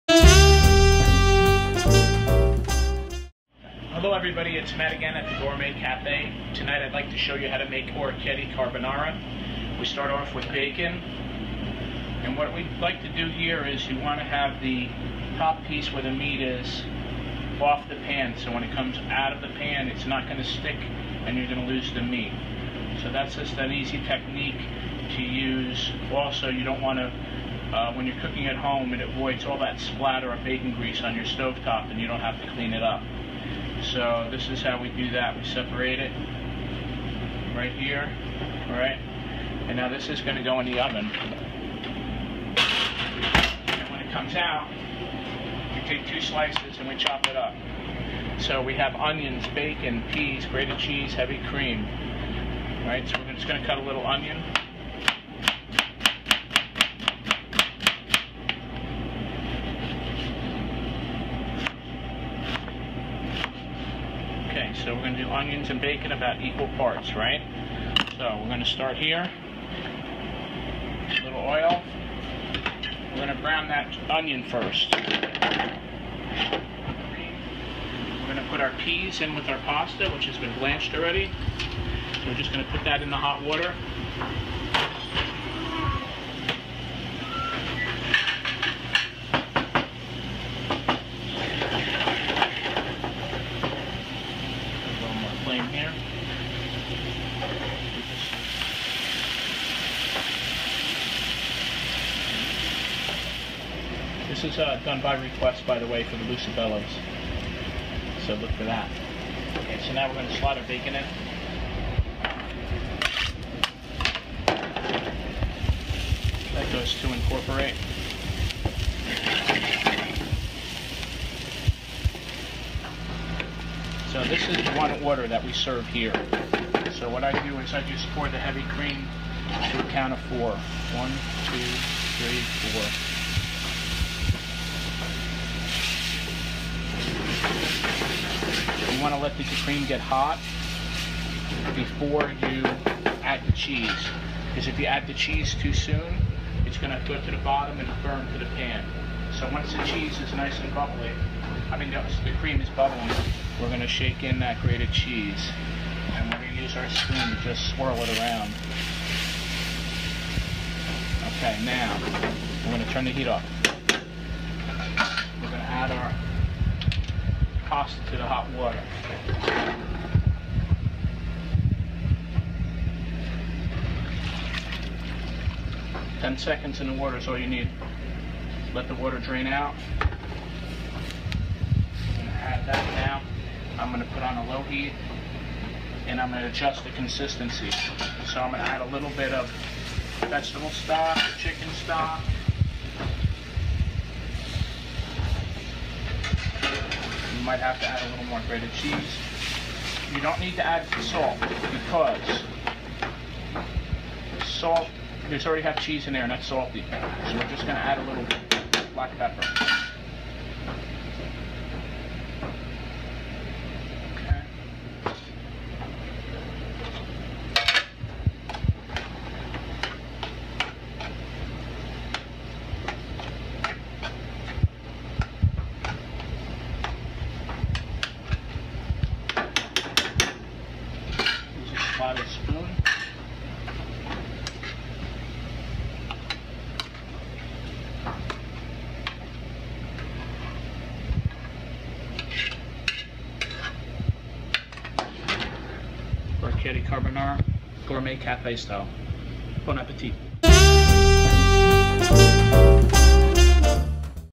Chef Matthew's Orecchiette Carbonara
Chef Matthew Pierone demonstrates how to prepare Orecchiette Carbonara. This dish is served at his restaurant the Gourmet Café in Parsippany NJ